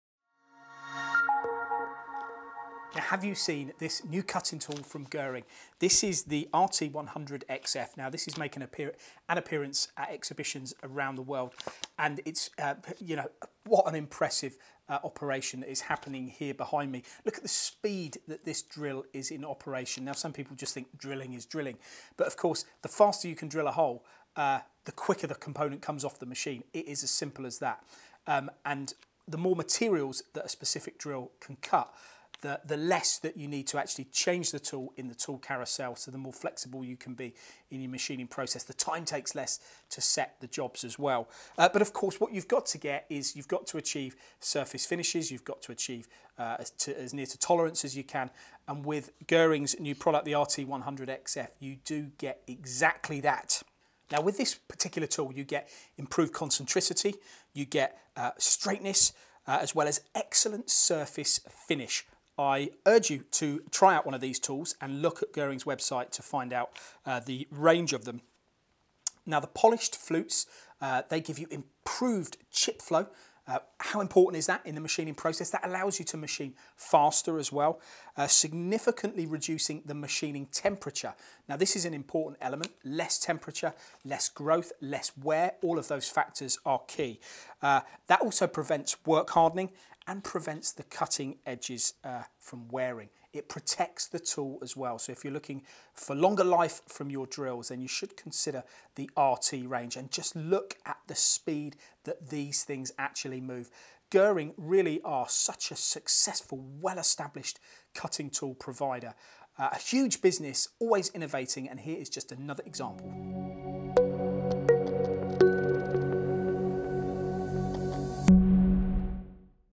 How fast is this drill, it's doing it dry to
Paul talks about the RT 100 from Guhring. Have you seen this drill in action? just watch the speed it moves and this is a possibility on all material types. Have you used one? give us your feedback, are they as good as they look?